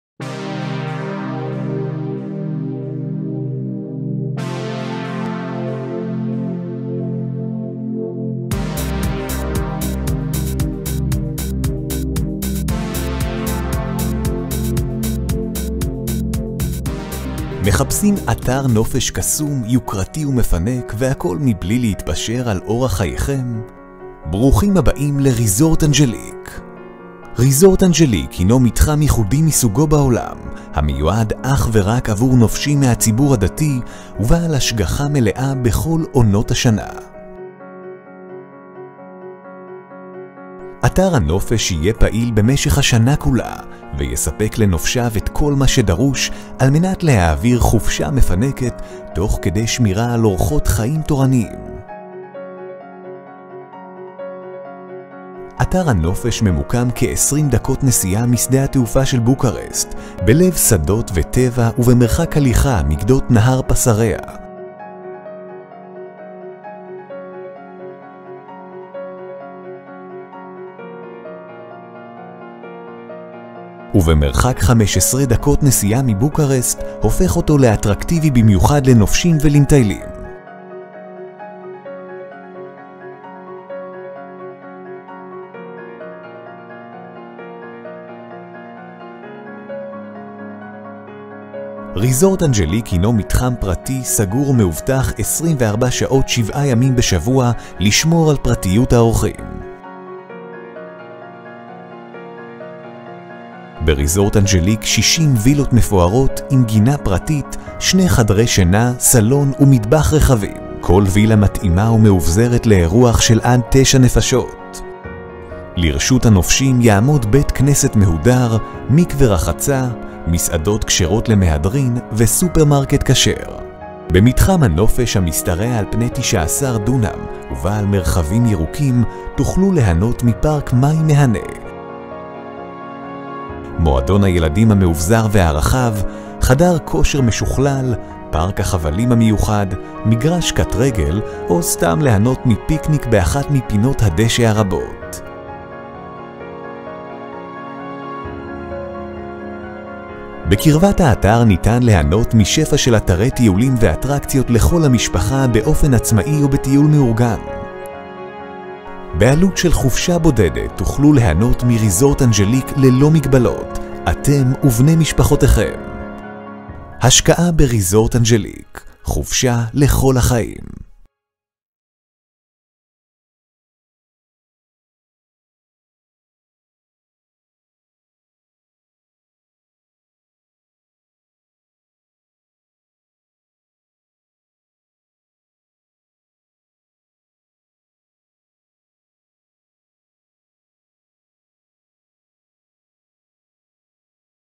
bPARK - כפר נופש למגזר החרדי בבוקרשט ובודשפט
חברת bPARK  מקימה אתרי נופש ייחודי ויעודי למגזר החרדי בלבד בבוקרשט ובודפשט. כל אתר משתרע על כ-19 דונם  וכולל כ-64 וילות מפוארות ומאובזרות, בית כנסת מפואר, מסעדות כשרות, סופרמרקט כשר, מקווה טהרה, מועדון ילדים, פארק מים מחוממים, מתקני שעשועים, אולם כנסים ועוד.